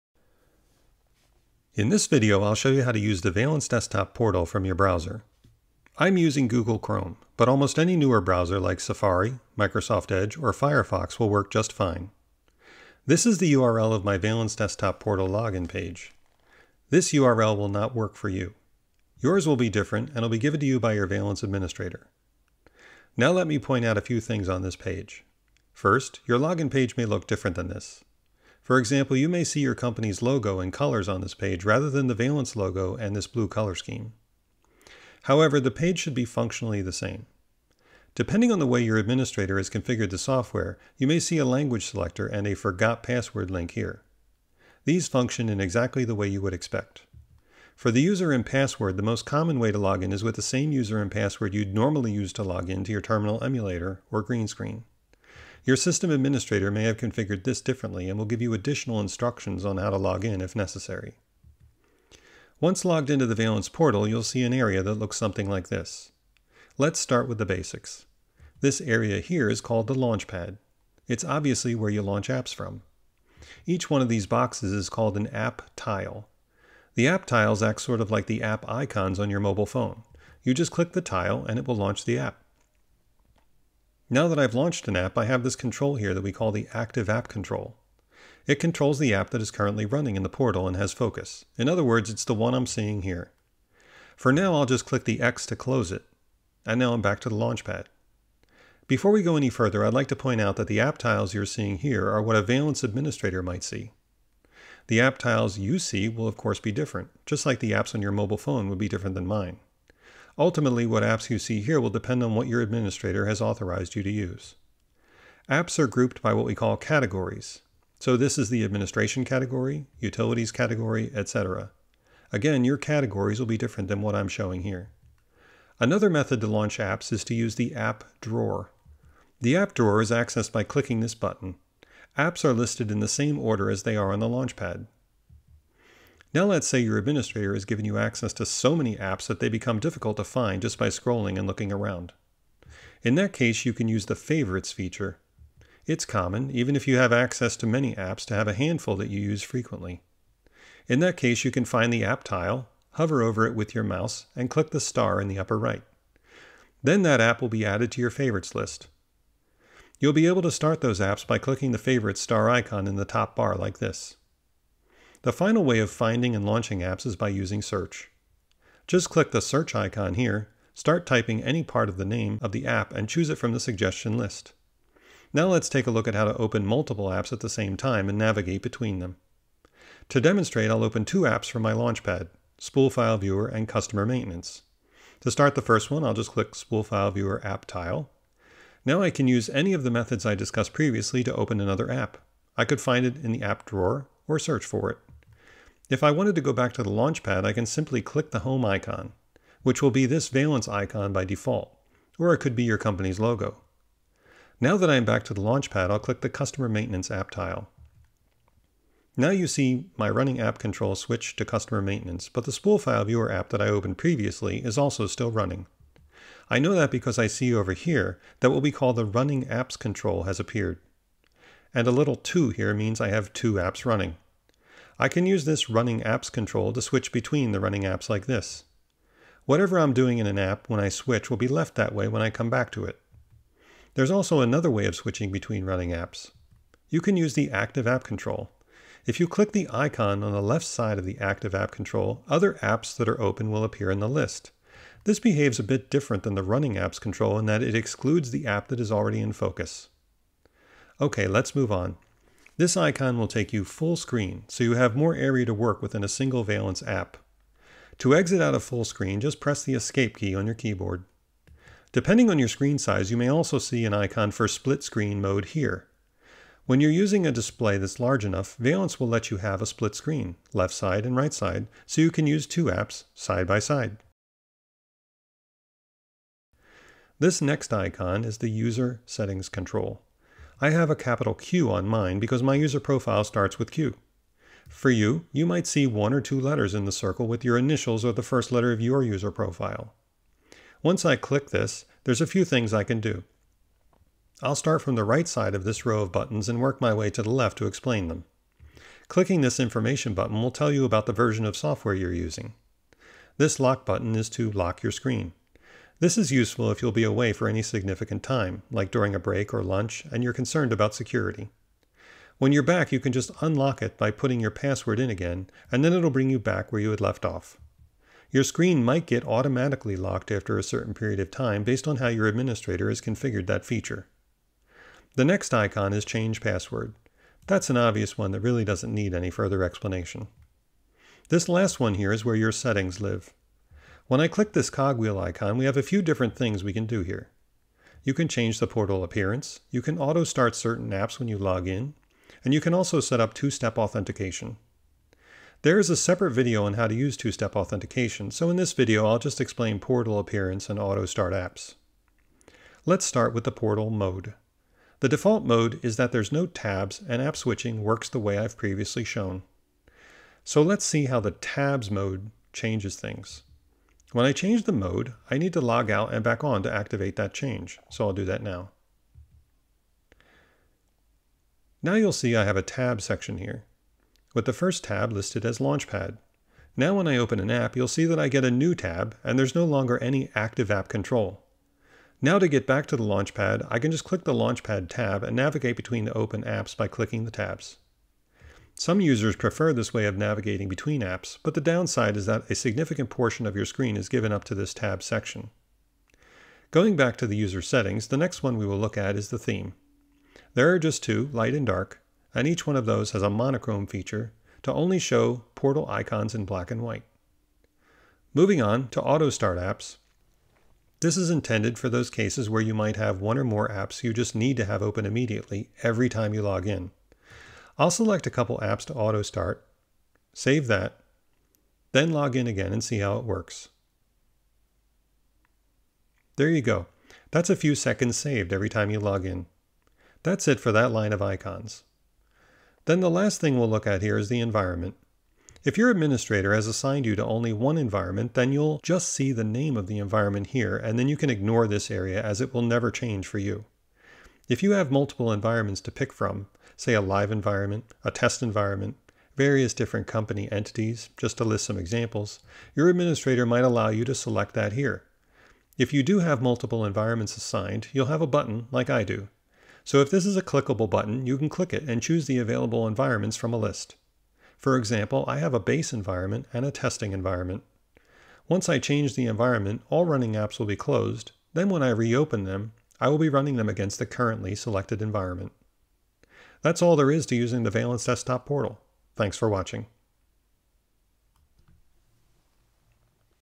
How to Use the Valence Desktop Portal
In this video learn the basics of how to use the Valence Desktop Portal, like logging in, finding and launching apps, switching between apps, user settings and more.

00:00 Intro and login page
01:01 Portal basics: launchpad, app tiles, active app control, categories, app drawer
02:15 Favorites
02:45 Search
02:54 How to open and navigate between multiple apps using running apps control and active app control
04:11 Full screen mode
04:20 Split screen mode
04:38 User settings: information panel, lock screen, change password
05:52 Portal mode: tabs or no tabs
06:37 Theme
06:47 Autostart apps
07:12 Discussion about environments
07:55 Log out